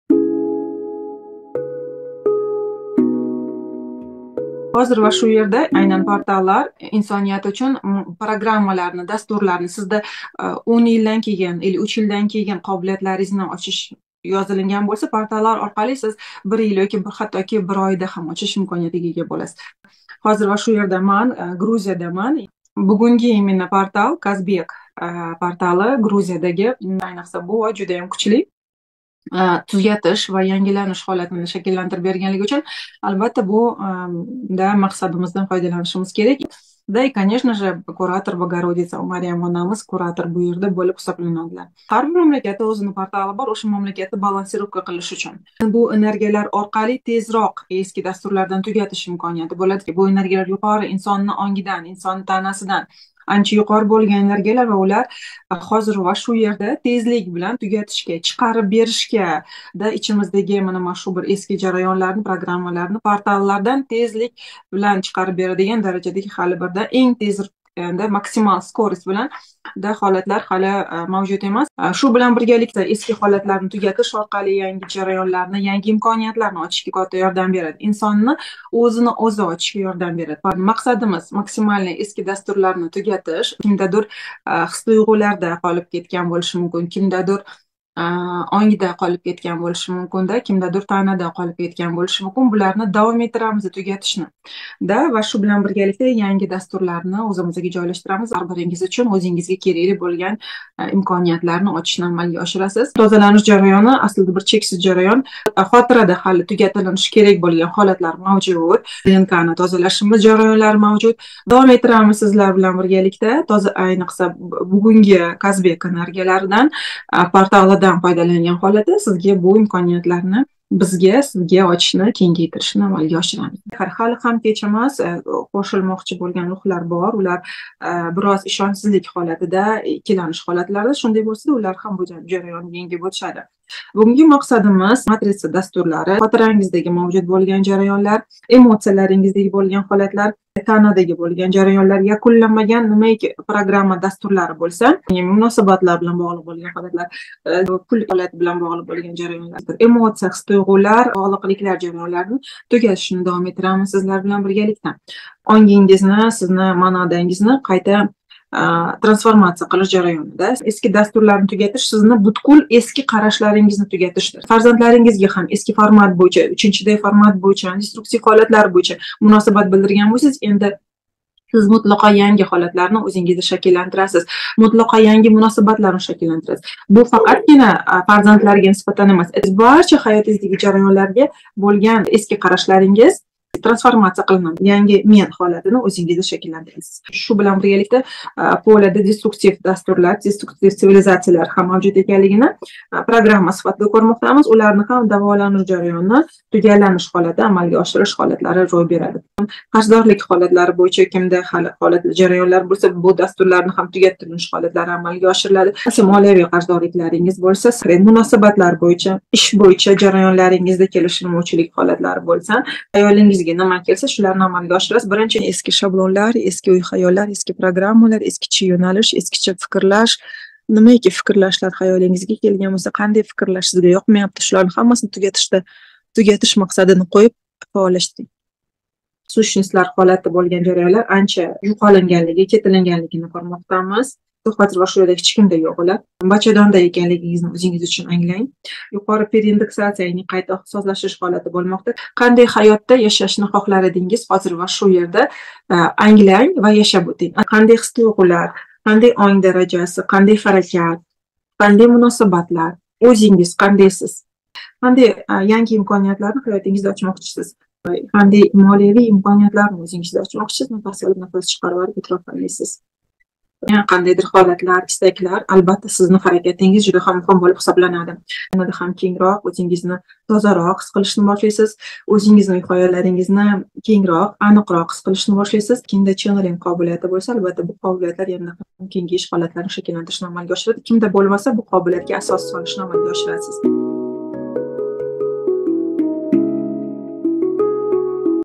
««ЯКУНЛАШ ВА ЯНГИЛАНИШ 999» «КАЗБЕК ЭНЕРГО ПОРТАЛИ»» жараёнлари...
🔔АЗИЗЛАР!!!🔔

⚛️««ЯКУНЛАШ ВА ЯНГИЛАНИШ 999» «КАЗБЕК ЭНЕРГО ПОРТАЛИ»»

Амалиёти жараёнларидан олинган ушбу видео оркали:

   ✅ Кучли тозаланиш жараёнлари оркали,

   ✅ Янги имкониятларнинг тезлик ила очилиши,

   ✅ Муносабатлардаги ва молиявий карздорликларни якунлаш,

   ✅ Эски шаблонлар оркали мавжуд булган хар кандай муносабатларни  сифатга олиб чикиш,

   ✅ "Юкори Онг - Высшее Я" билан богланиш холатини янада кучайтириб,

   ✅ Ченнелинг каналини очиш имкониятларини кенгайтириб бериши

тугрисида билиб олишингиз мумкин булади🙏🙏

‼️Эслатиб утамиз Азизлар,  ушбу амалиёт видео дарсидан фойдаланган холда, узингиз учун кенг микиёсдаги имкониятларни ишга тушира олишингиз мумкин🙏💜

‼️УНУТМАНГ!!!  ИМКОНИЯТЛАР ЧЕКСИЗ ВА ЧЕГАРАЛАНМАГАН УЛАРНИ БИЗ ЯРАТАМИЗ!!!

Мурожаат учун 👇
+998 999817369

🦋 РИБАСА 369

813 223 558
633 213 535